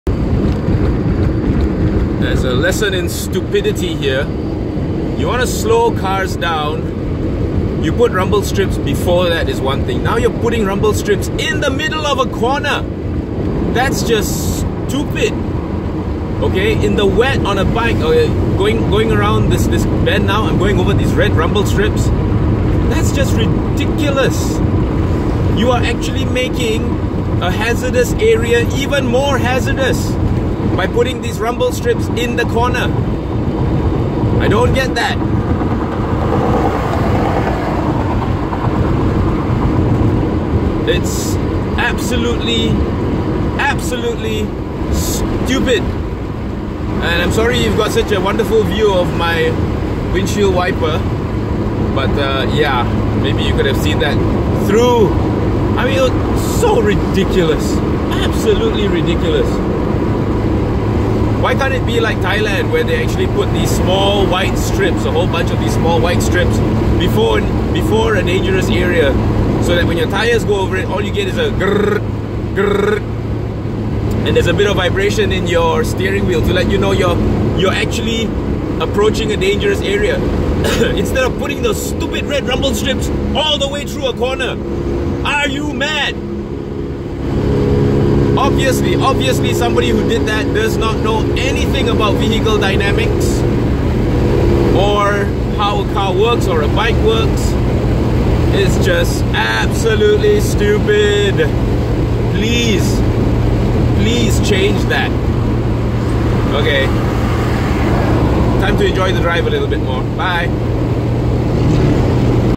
Lesson in Stupidity: Making a Hazardous Area Even MORE Dangerous!! | EvoMalaysia.com
YOU KNOW folks, I actually tried to start the video earlier during the FIRST SET of Red rumble-strips at Changkat Jering, but the jarring was SO BAD the camera would not start because it couldn't focus!! Yes, it's THAT ridiculous.

Whoever designed this so-called road "safety system", u sir are a freaking moron who has NO clue how to make a road safer. You've actually made a Hazardous area EVEN MORE HAZARDOUS!! And these strips will damage a cars' & bikes' suspension components for sure, wheels and tyres too. Who's gonna pay for that? YOU??

OK, rant over. What do u think folks? Is this rant justified? Have your say in the comments!!